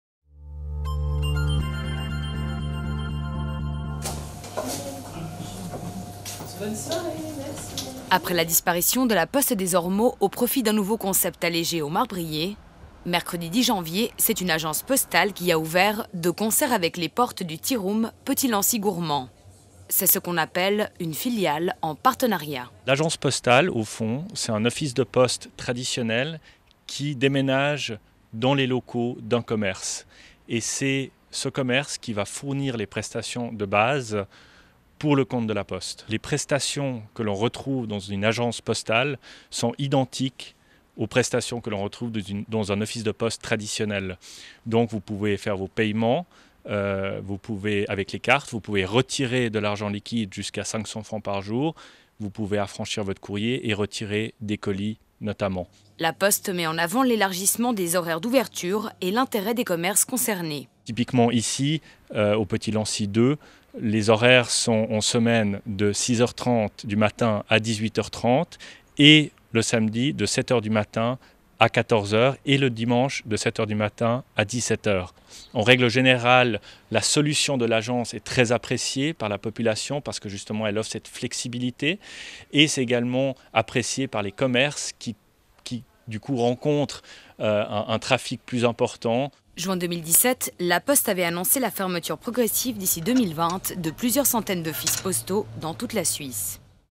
LANCY TV - Les Marbriers: adieu la Poste, bonjour l'agence postale !
Les Marbriers: adieu la Poste, bonjour l'agence postale !